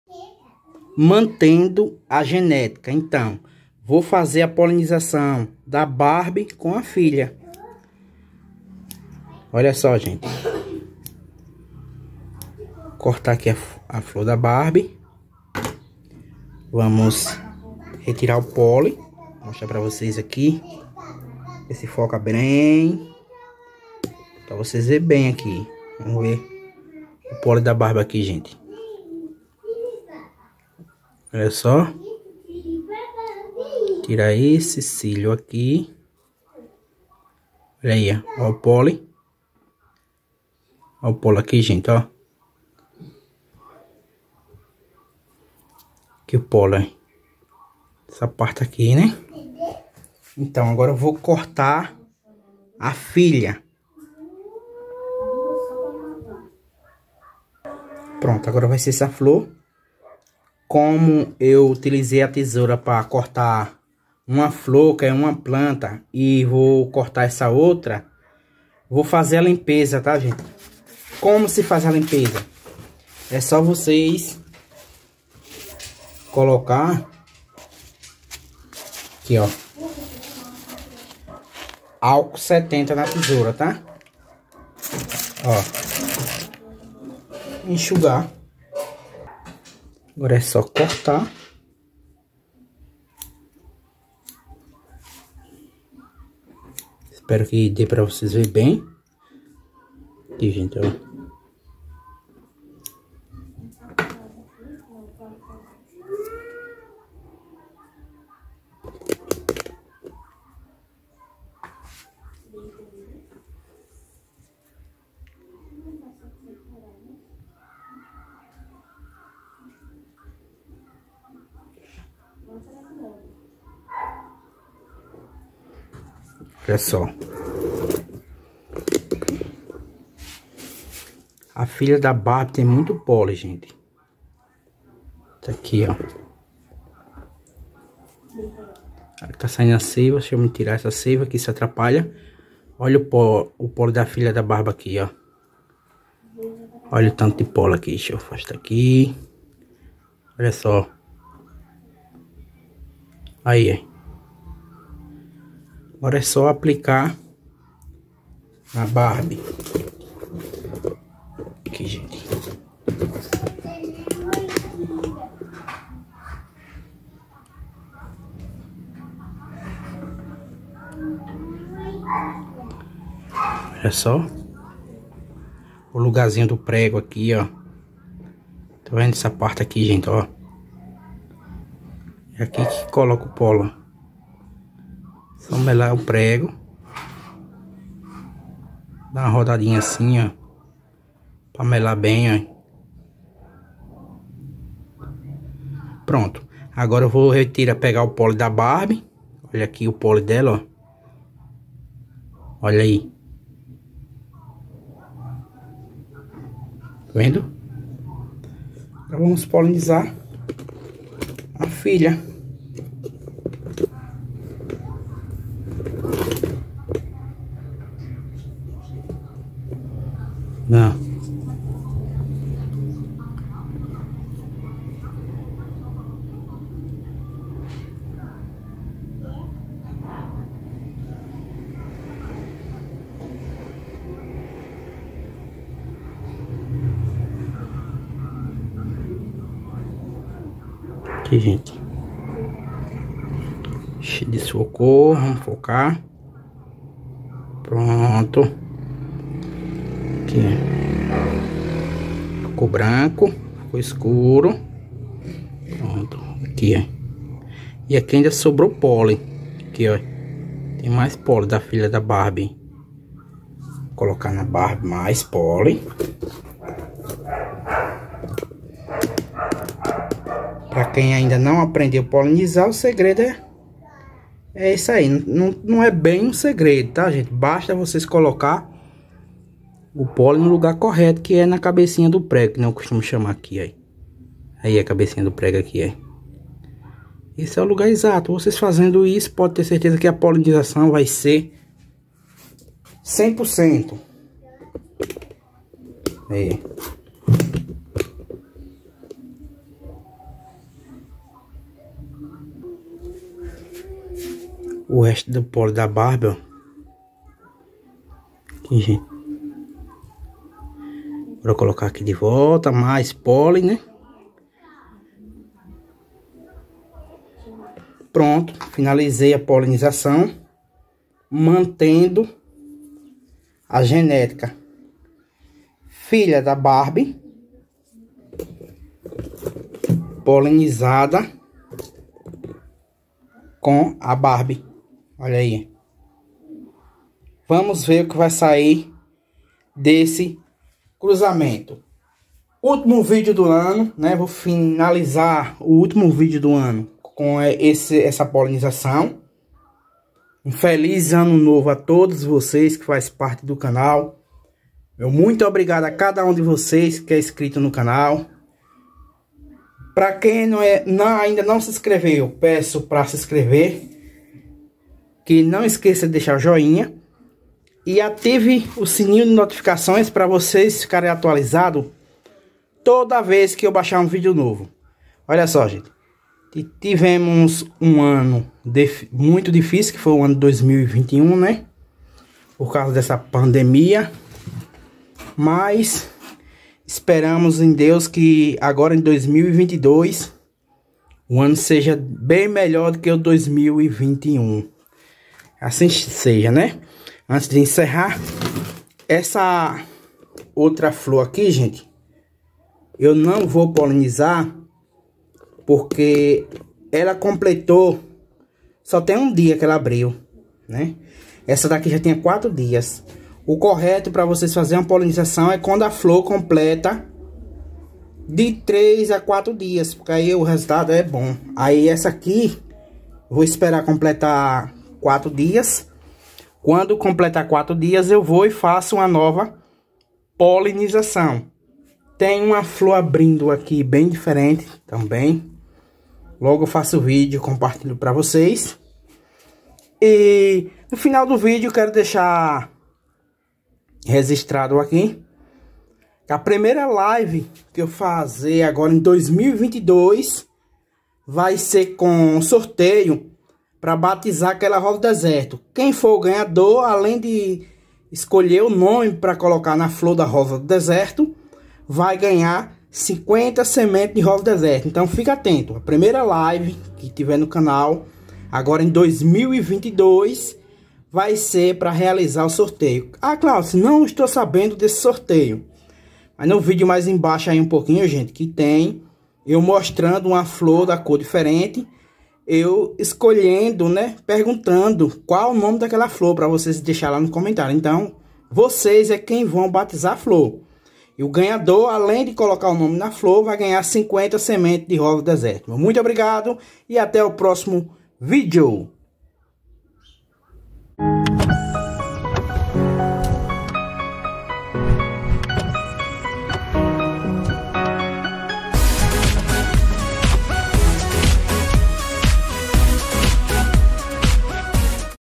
melhorando a genética da rosa do deserto com a polinização
aprenda como manter a genética da rosa do deserto, nesse vídeo eu mostro.